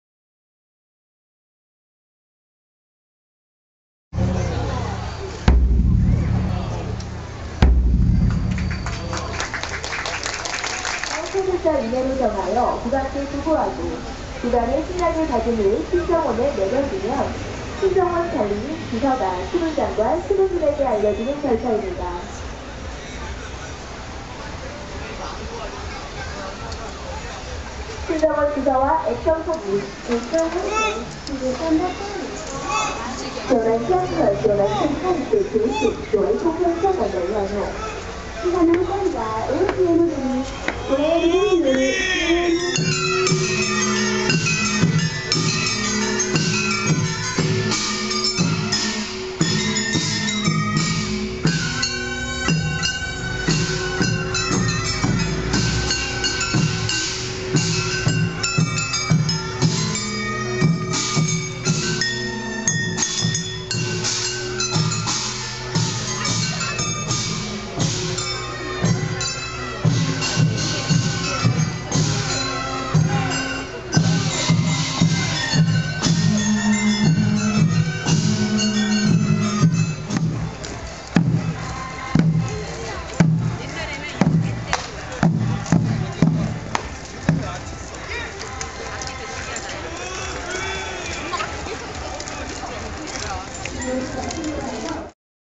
Changing of the guards at Deoksugung (덕수궁)
Changing of the guards in front of Deoksugung in Seoul (서울). Deoksugung (덕수궁) is one of the five Joseon palaces in Seoul. It is located in front of Seoul city hall on the road the Gwanghwamun.